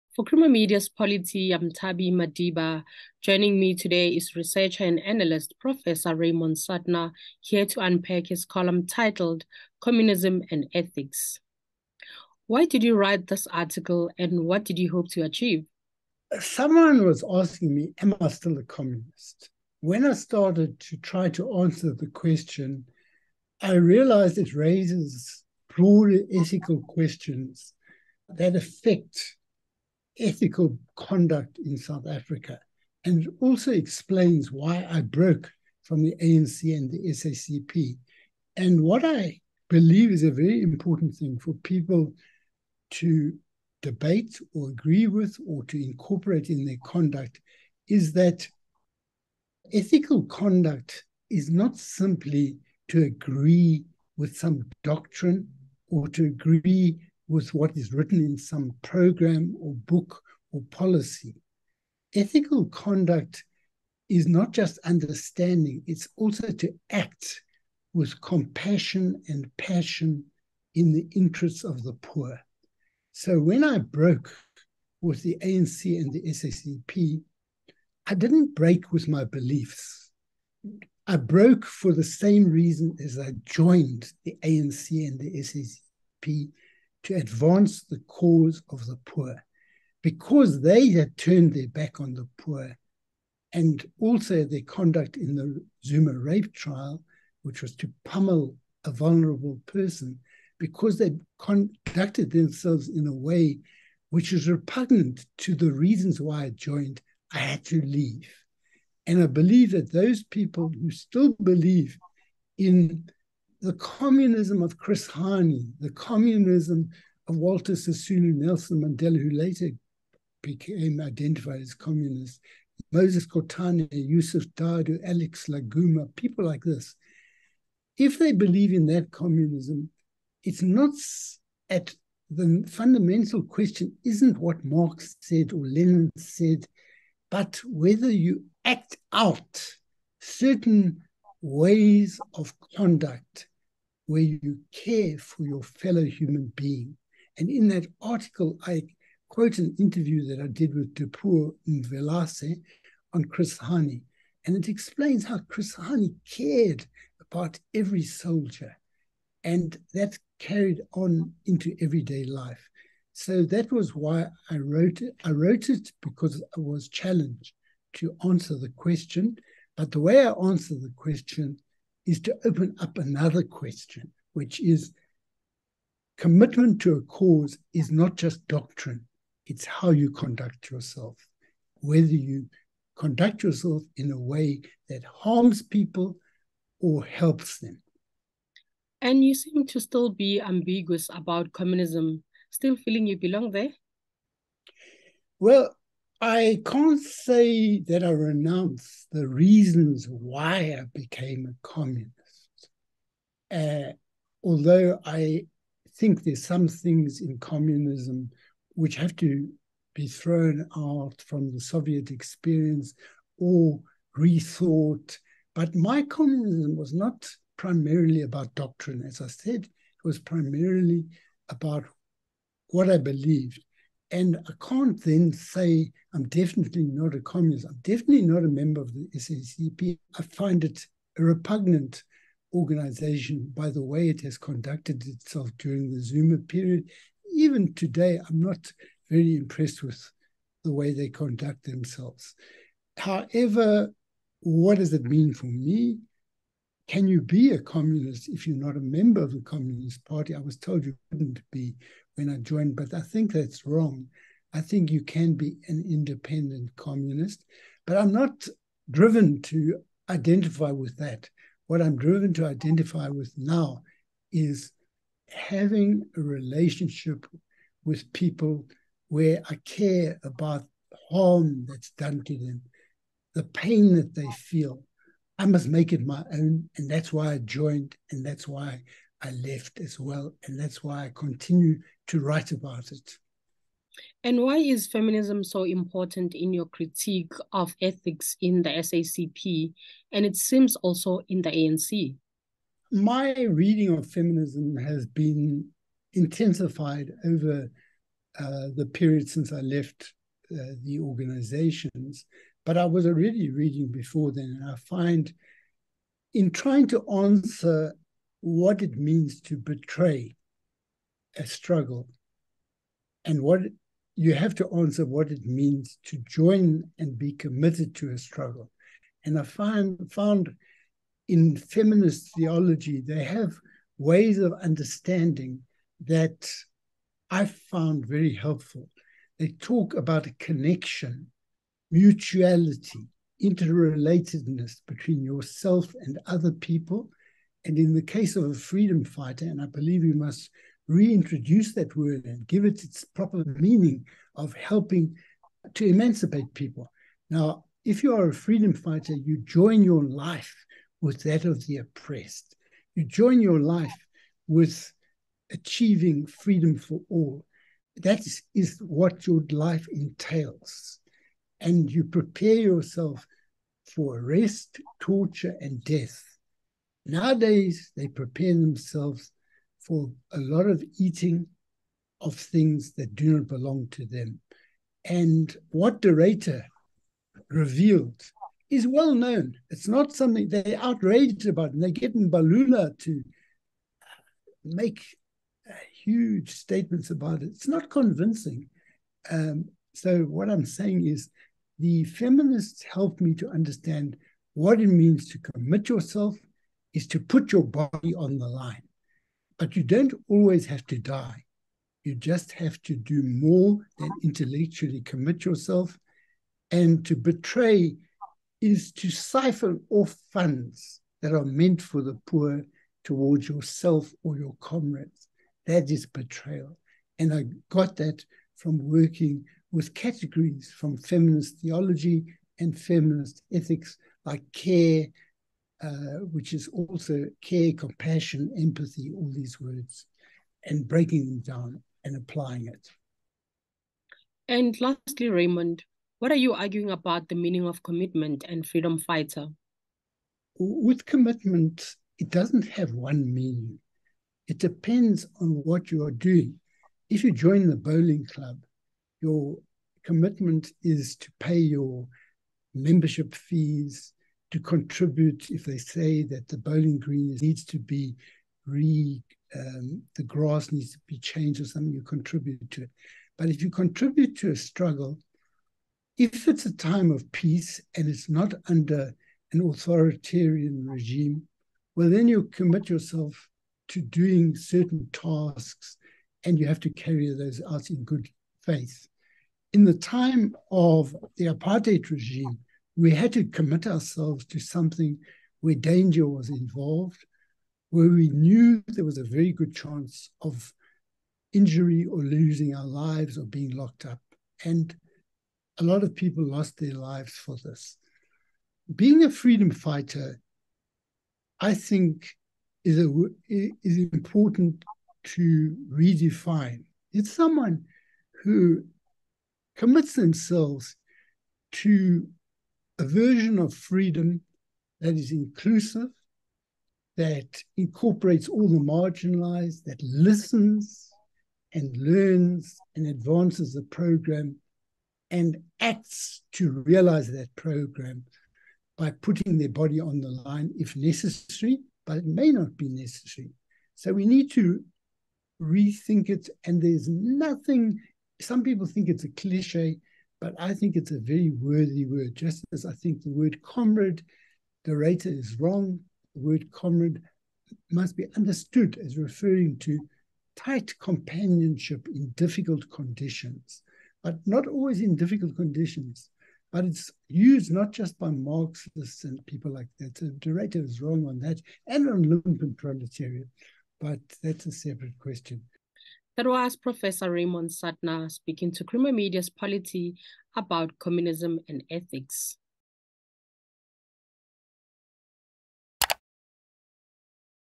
Raymond Suttner on: Being or not being a communist in South Africa today
This week, Professor Raymond Suttner discusses "Being or not being a communist in South Africa today".

From 1969/70 until 2006 I had been a fairly orthodox member of the South African Communist Party, and I broke ranks over the party’s conduct during the Jacob Zuma rape trial and its collusion in the corruption of the years of his state presidency, dramatically illustrated in the diversion of funds intended for poverty relief towards refurbishing Zuma’s Nkandla home into a luxurious mansion. I have had no official contact since then and very little interaction with individuals who remain members, he writes.